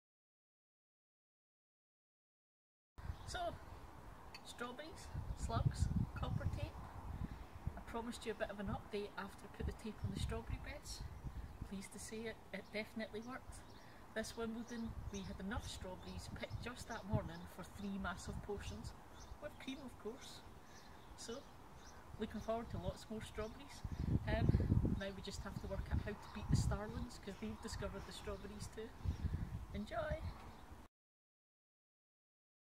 Did the copper tape beat the slugs and save my strawberries?
An update on my ongoing battle to stop the slugs from eating all my strawberries.
Did putting copper tape around the raised bed work? .